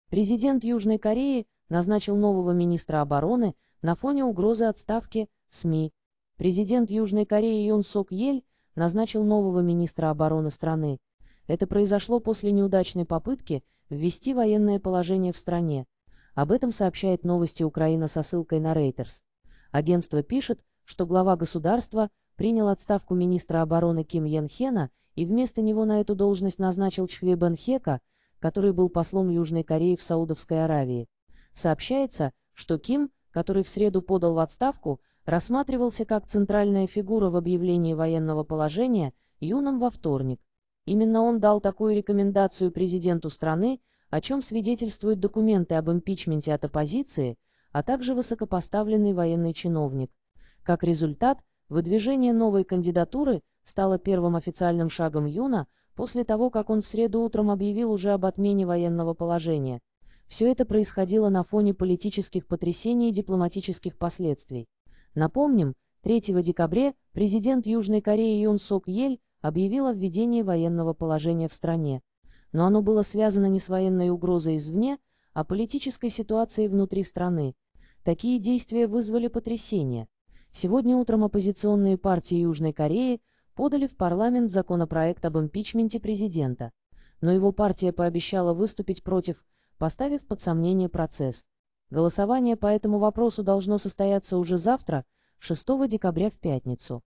Президент Южной Кореи назначил нового министра обороны на фоне угрозы отставки, - СМИ.
ЮжнаяКорея Президент Президент Южной Кореи Юн Сок Ель назначил нового министра обороны страны. Это произошло после неудачной попытки ввести военное положение в стране. Об этом сообщает Новости Украина со ссылкой на Reuters. Агентство пишет, что глава государства принял отставку министра обороны Ким Ен Хена и вместо него на эту должность назначил Чхве Бен Хека, который был послом Южной Кореи в Саудовской Аравии. Сообщается, что Ким, который в среду подал в отставку, рассматривался как центральная фигура в объявлении вое